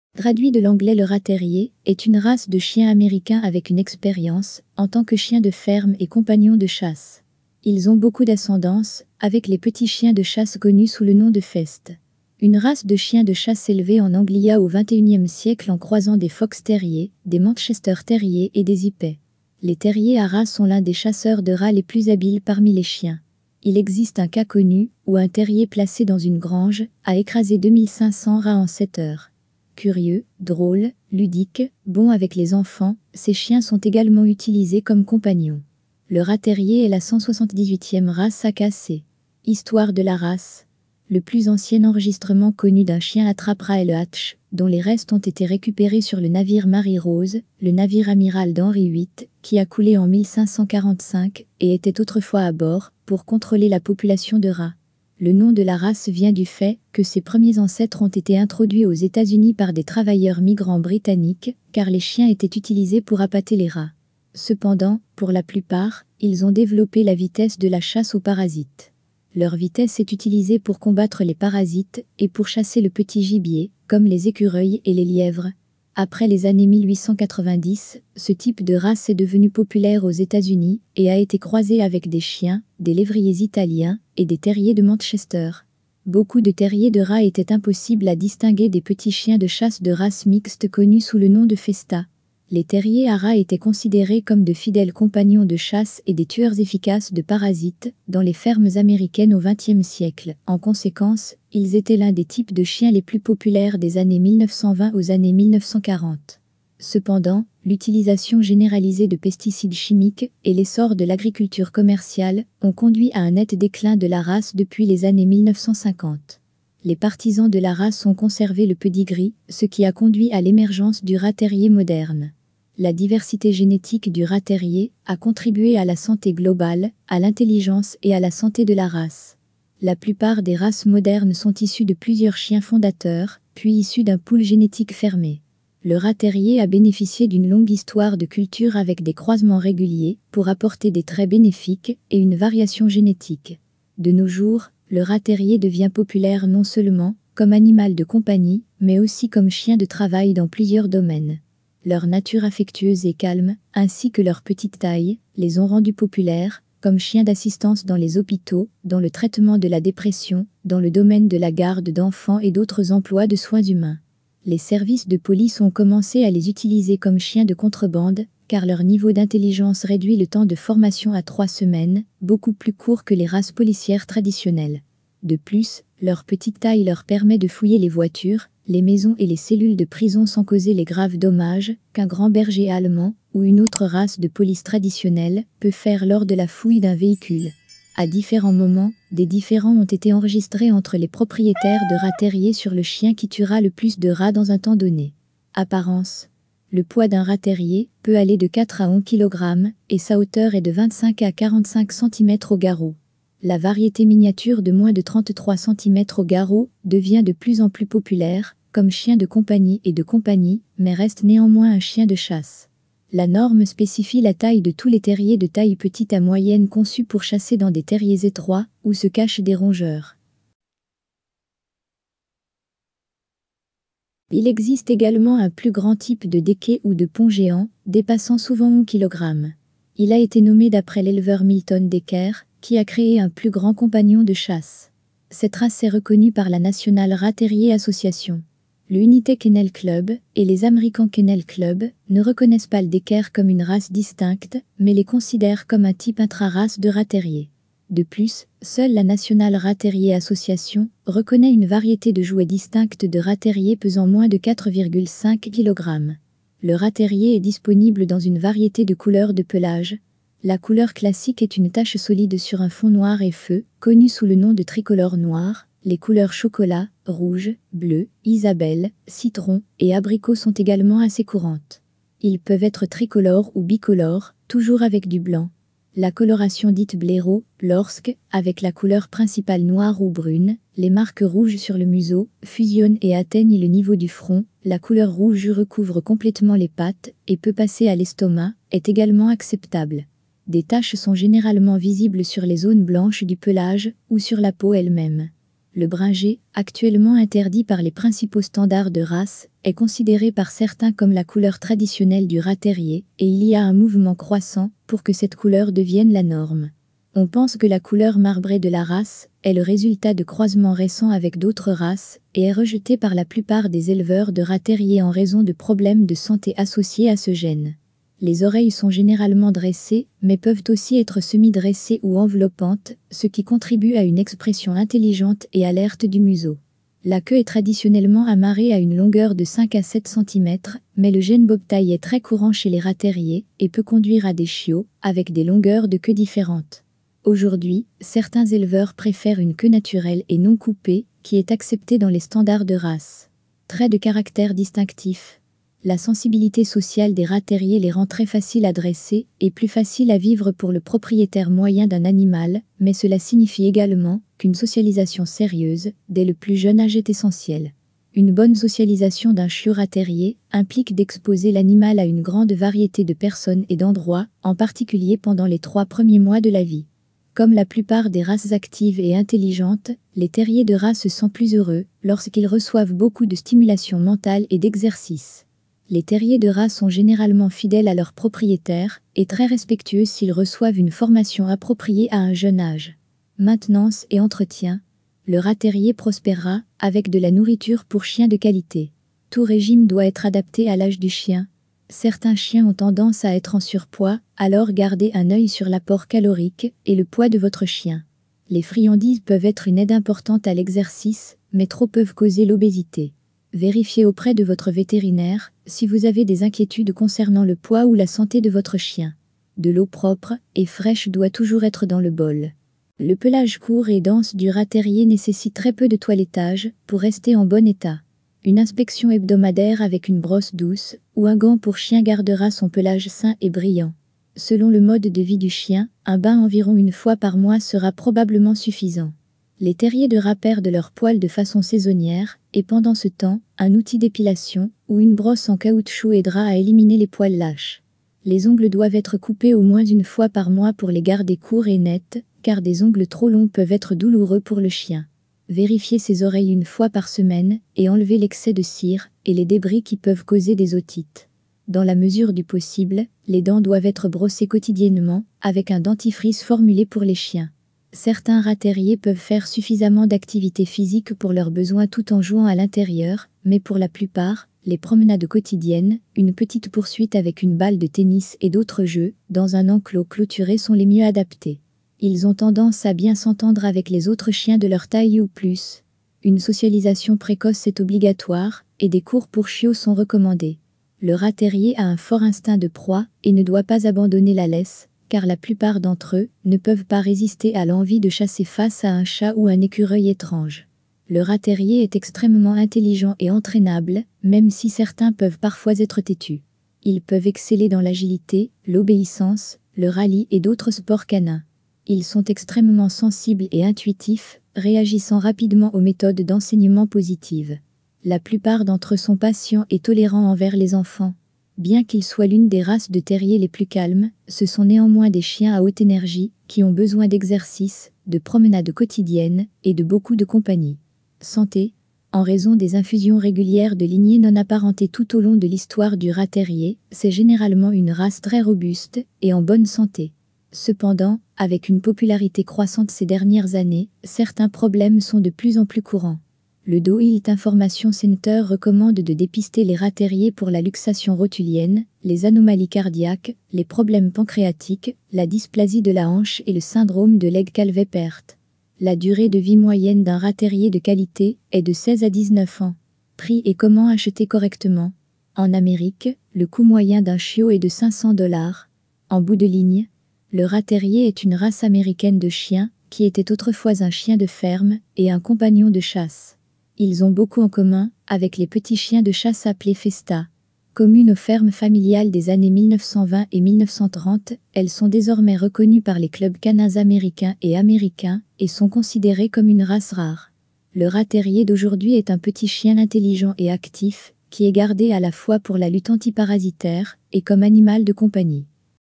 Rat Terrier. Avantages et inconvénients, Prix, Comment choisir, Faits, Soins, Histoire
==►Le plus ancien enregistrement connu d'un chien attrape-rats est le Hatch, dont les restes ont été récupérés sur le navire Mary Rose, le navire amiral d'Henri VIII qui a coulé en 1545 et était autrefois à bord pour contrôler la population de rats.

==►Thank you very much:
Merci bien:

"Charlotte, NC - Blue Rat Terrier, Noel" by etherealist
"Miep, Rat Terrier" by nijin
"Charlotte, NC - Blue Rat Terrier, Noel" by Desiree Kane
"Wyatt the Rat Terrier loves the birds" by imwilliam
"rat-terrier-76942_1280" by localpups
"Chiquita (106/365)" by LifeSupercharger
"Tuffy2007-1" by DirectDish
"Tired little boy" by Dusty J
"Chiquita (34/365)" by LifeSupercharger
"Dog" by DirectDish
"Pause for Santa Impersonation" by greeblie
"Posing in the Sunlight" by greeblie
"Chiquita taking a nap" by LifeSupercharger
"Huntington Dog Beach 2010" by LifeSupercharger
"Huntington Dog Beach" by LifeSupercharger
"Happy Dog!" by greeblie
"Sad Trixie" by txbowen
"Chiquita posing" by LifeSupercharger
"Are you following me?" by LifeSupercharger
"Tuffy2007-2" by DirectDish
"Close-up of Chiquita" by LifeSupercharger
"Dog Doesn't Like Being Eyeballed" by greeblie
"Ziggy 2" by justinplambert
"File:Lucy, a Rat Terrier.jpg" by Michael Geary
"On the prowl" by LifeSupercharger
"Chiquita soaking in the SoCal sun" by LifeSupercharger
"My dog Frank" by Dusty J
"Kiko" by Pedro Lozano
"Alarm Dog On Duty" by greeblie
"DSCF0393" by DirectDish
"Puppy Dreams" by greeblie
"Sooo Sleepy" by txbowen
"miep profile" by nijin
"DSC00075" by mezmer1
"miep 4 months" by nijin